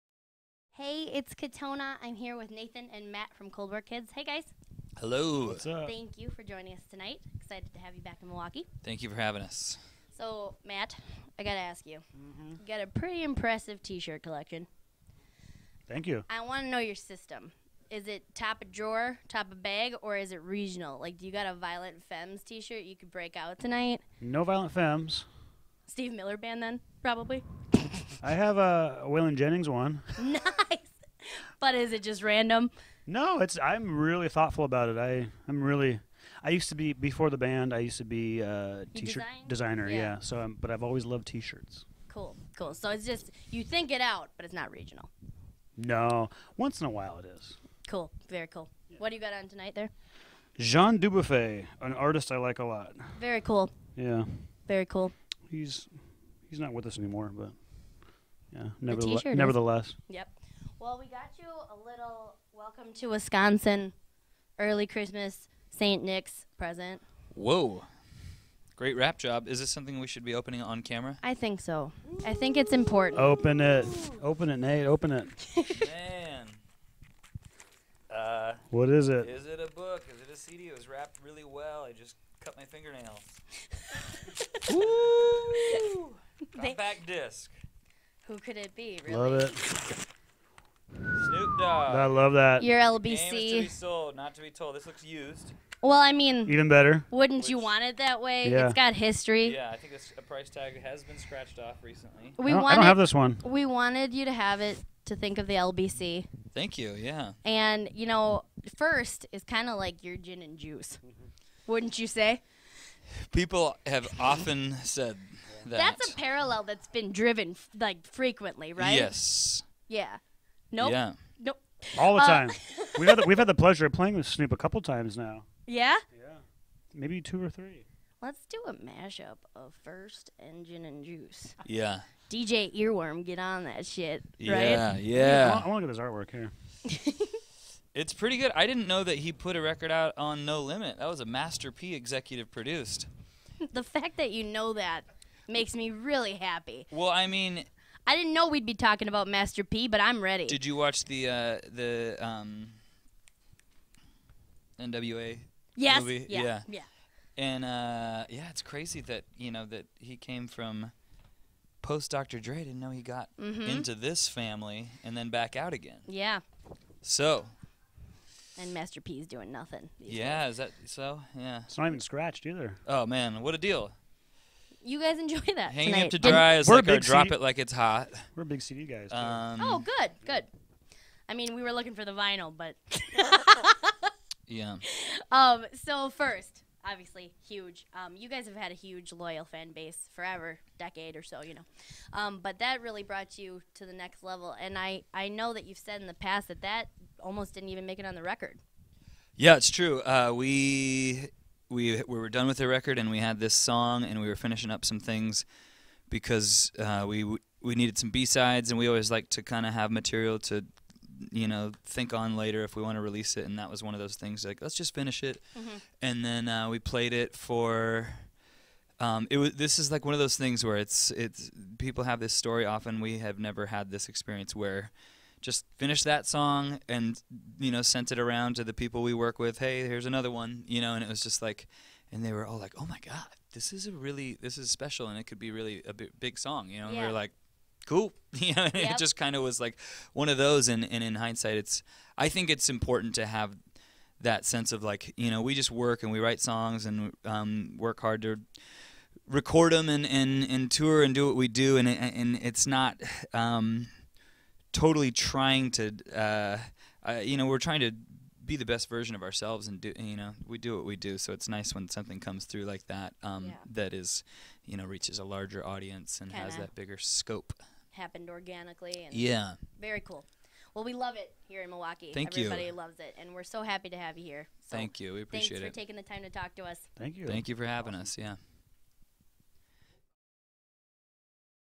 FM 102/1 BSSX Interview: Cold War Kids
We love Snoop Dogg. So do Cold War Kids. We discuss our mutual affection for the D-O-Double-G before their set at BSSX at Eagles Ballroom in Milwaukee December 11, 2015.